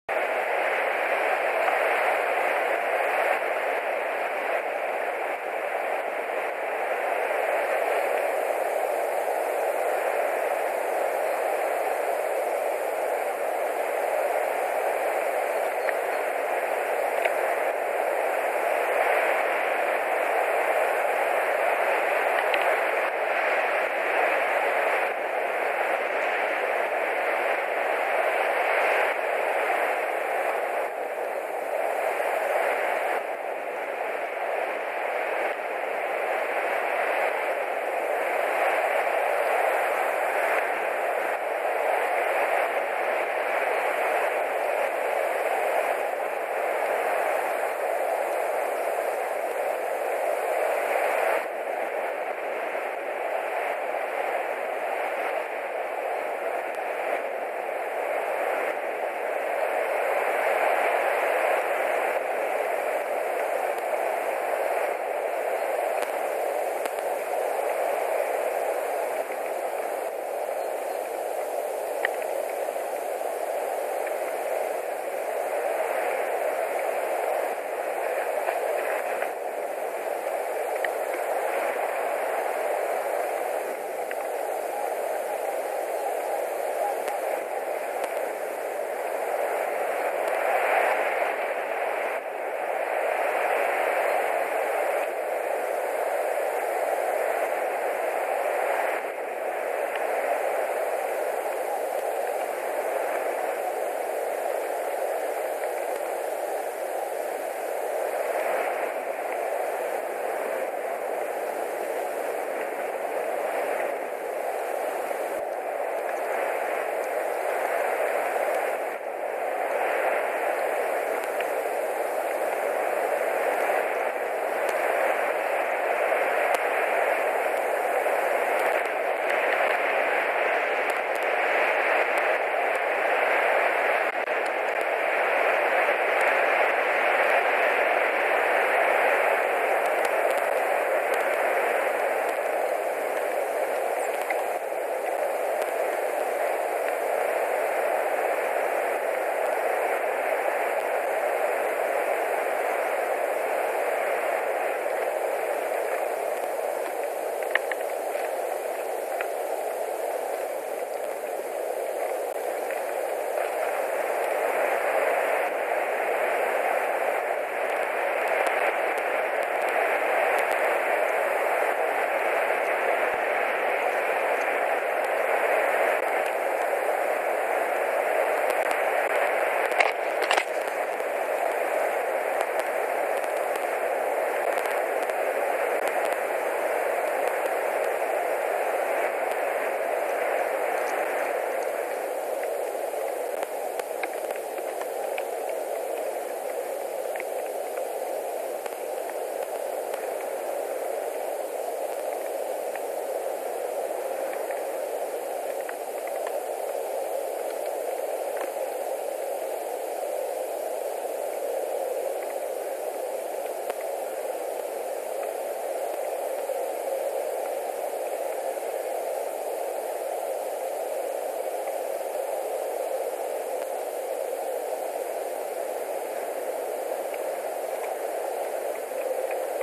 White-tailed Eagles-Haliaeetus albicilla-Latvia nest 2021 03 09 11:02 Raims with straw
11:02 Raims came with straw
12:10 flew away